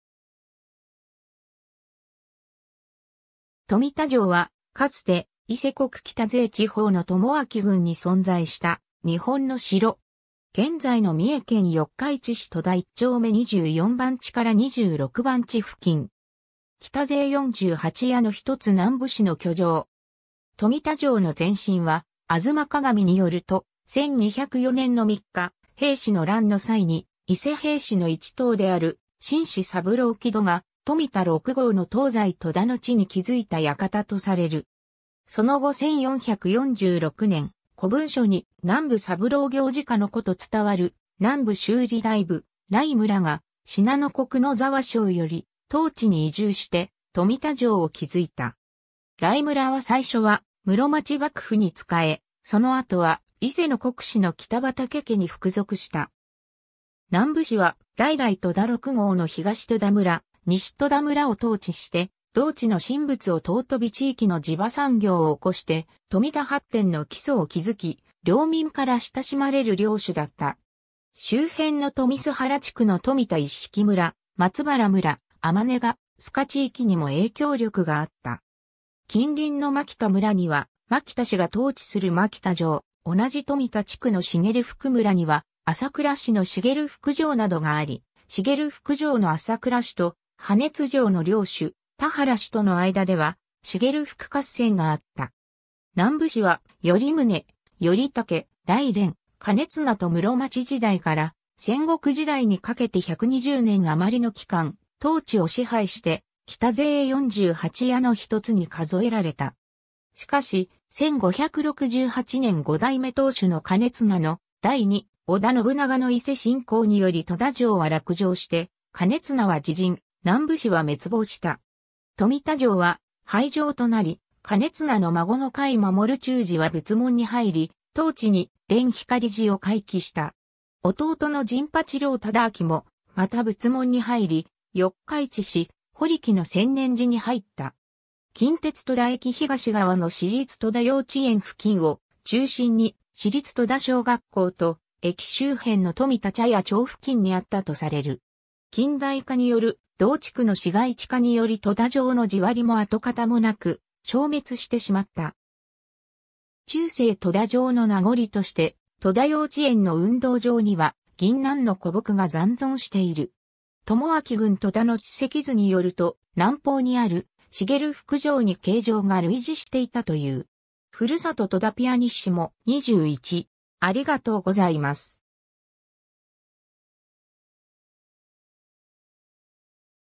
富田城 (伊勢国)
富田_(四日市市)
富田城（とみだじょう）は、かつて伊勢国北勢地方の朝明郡に存在した日本の城。
現在の三重県四日市市富田1丁目24番地～26番地付近。
北勢四十八家の1つ南部氏の居城。
 富田城の前身は『吾妻鏡』によると、1204年（元久元年）の三日平氏の乱の際に伊勢平氏の一党である進士三郎基度が、富田六郷の東西富田の地に築いた居館とされる。
その後1446年（文安3年）、古文書に南部三郎行直の子と伝わる南部修理大夫頼村が、信濃国野沢庄より当地に移住して富田城を築いた。
頼村は最初は室町幕府に仕え、その後は伊勢の国司の北畠家に服属した。
 南部氏は、代々富田六郷の東富田村・西富田村を統治して、同地の神仏を尊び地域の地場産業を興して富田発展の基礎を築き、領民から親しまれる領主だった。
周辺の富洲原地区の富田一色村・松原村・天ヶ須賀地域にも影響力があった。
近隣の蒔田村には蒔田氏が統治する蒔田城 (伊勢国)、同じ富田地区の茂福村には朝倉氏の茂福城などがあり、茂福城の朝倉氏と羽津城の領主・田原氏との間では茂福合戦があった。
 南部氏は頼宗、頼武、頼連、兼綱と室町時代から戦国時代にかけて120年余りの期間、当地を支配して北勢四十八家の一つに数えられた。
しかし、1568年（永禄11年）5代目当主の兼綱の代に、織田信長の伊勢侵攻により富田城は落城して兼綱は自刃、南部氏は滅亡した。
富田城は廃城となり、兼綱の孫の甲斐守忠次は仏門に入り、当地に蓮光寺を開基した。
弟の甚八郎忠明もまた仏門に入り四日市市堀木の専念寺に入った。
 近鉄富田駅東側の市立富田幼稚園付近を中心に、市立富田小学校と駅周辺の富田茶屋町付近にあったとされる。
近代化による同地区の市街地化により富田城の地割も跡形もなく消滅してしまった。
中世富田城の名残として富田幼稚園の運動場には銀杏の古木が残存している。
朝明郡富田の地籍図によると、南方にある茂福城に形状が類似していたという。
 ^ 『ふるさと富田』pp.21（1～9行目）